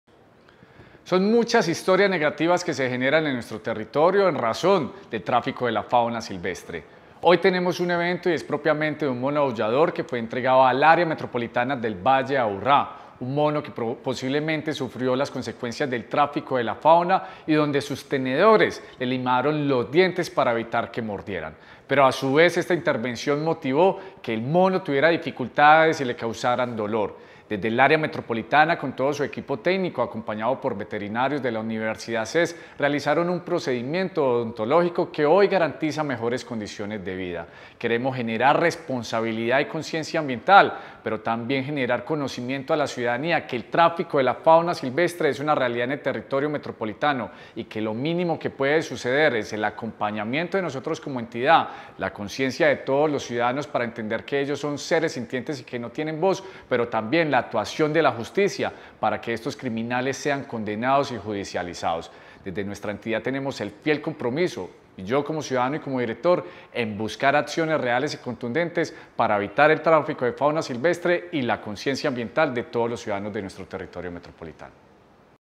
Aullador fue operado en Medellín
Juan David Palacio Cardona, director del Área Metropolitana del Valle de Aburrá, explicó que tras el exitoso procedimiento, el mono aullador se recupera satisfactoriamente en el Centro de Atención y Valoración de Fauna del Área Metropolitana del Valle de Aburrá, operado en convenio con la Universidad CES.